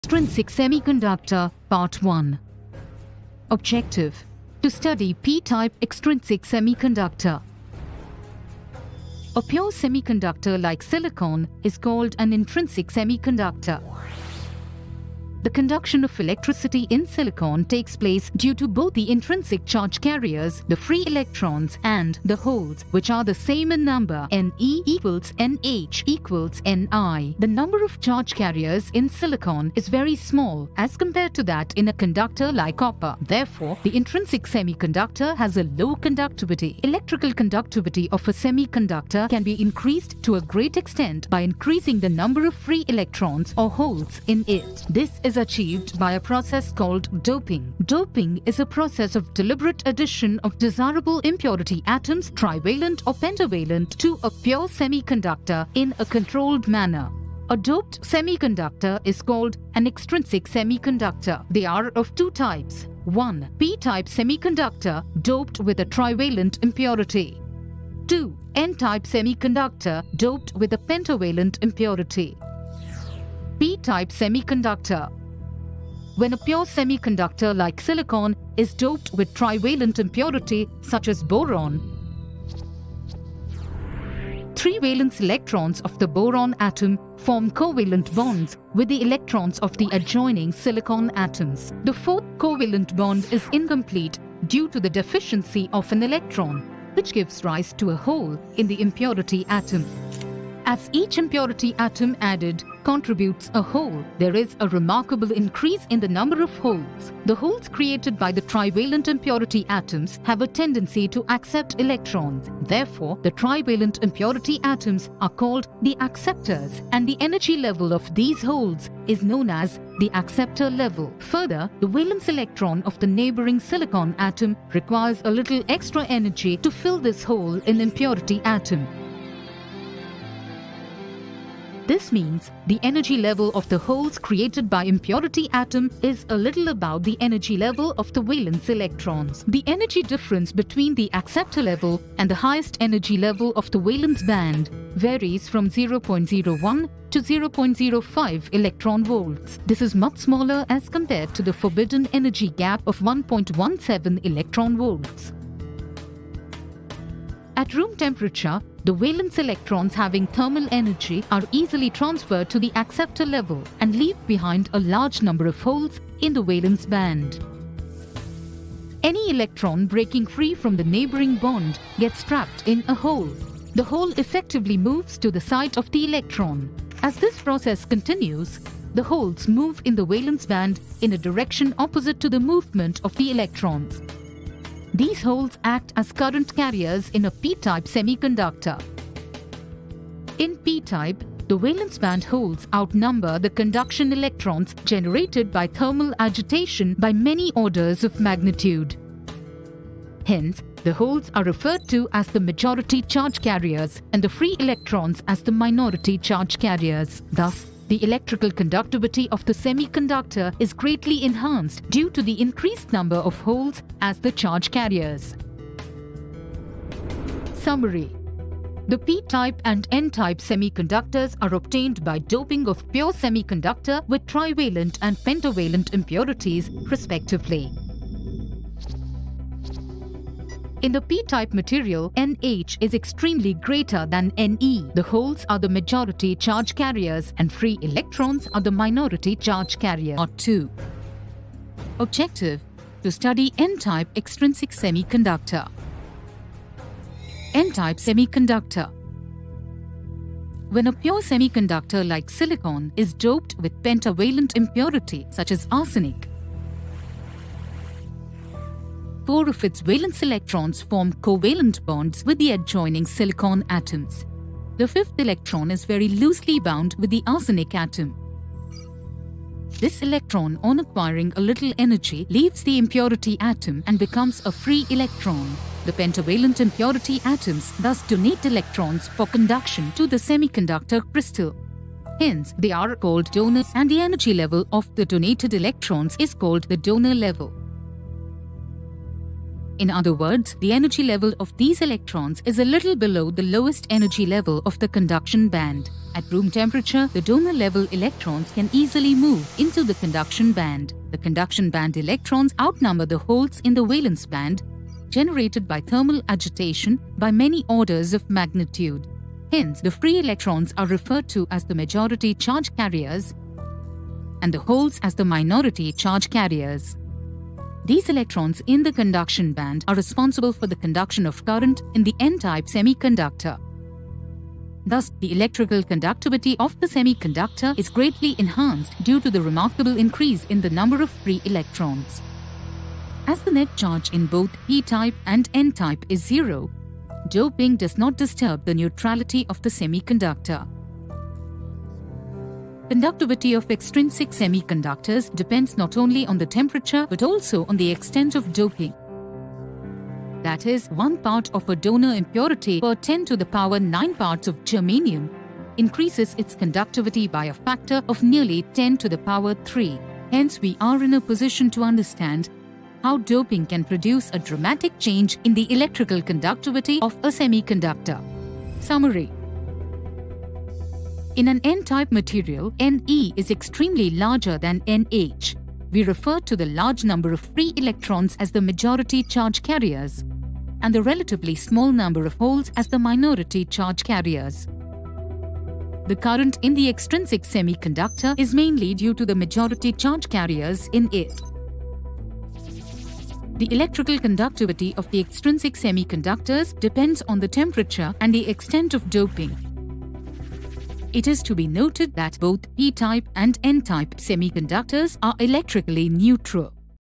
Episode 2 | Extrinsic semiconductors | Extrinsic semiconductors animation
"Extrinsic Semiconductors"

*Title:* "Extrinsic Semiconductors Explained: Doping, Types, and Applications"

*Description:*

"Unlock the secrets of extrinsic semiconductors! In this video, we dive into the world of doped semiconductors, exploring their properties, types, and real-world applications.

*Topics Covered:*

1. Introduction to Extrinsic Semiconductors
2. Doping Process: Donors and Acceptors
3. Types of Extrinsic Semiconductors:
    - N-Type Semiconductors
    - P-Type Semiconductors
4. Applications:
    - Electronic Devices (Transistors, Diodes)
    - Solar Cells
    - Optoelectronics
5. Comparison with Intrinsic Semiconductors

*Key Takeaways:*

- Understand the concept of doping and its effects
- Learn about N-Type and P-Type semiconductors
- Discover the various applications of extrinsic semiconductors

*Timestamps:*

0:00 - Introduction
2:30 - Doping Process
6:45 - N-Type Semiconductors
11:10 - P-Type Semiconductors
16:30 - Applications
22:50 - Conclusion

*Comment* below with your questions or suggestions for future videos.

- Extrinsic Semiconductors
- Doped Semiconductors
- N-Type Semiconductors
- P-Type Semiconductors
- Electronic Devices
- Solar Cells
- Optoelectronics
- Semiconductor Physics